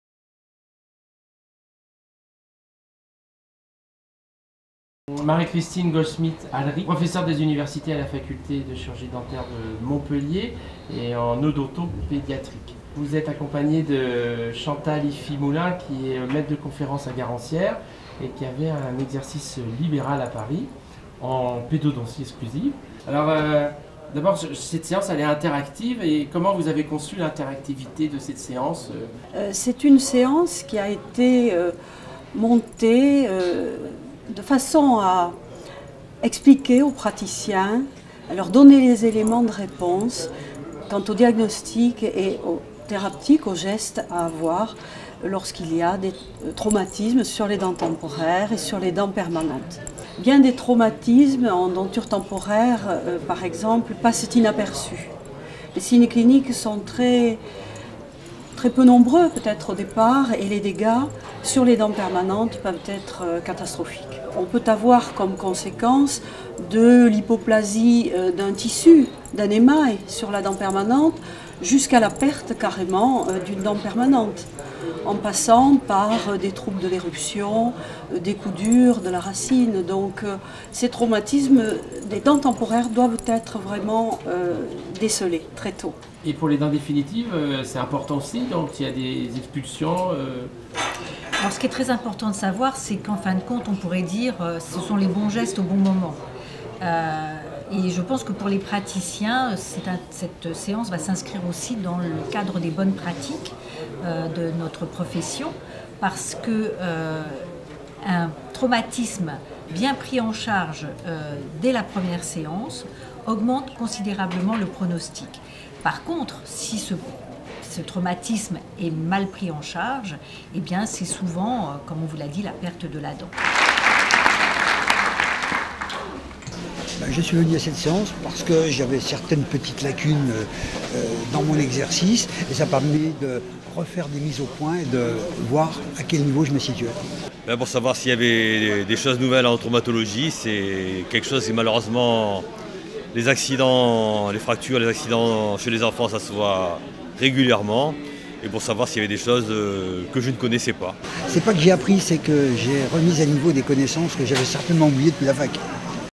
Congrès ADF 2012 - 27/11 : Séance interactive (A10) : Les traumatismes dentaires
En direct des salles de conférences : Séance interactive (A10) : Les traumatismes dentaires : Questions à Marie-Christine Goldsmith Alric, Chantal Ifi-Naulin et aux congressistes assistant à la séance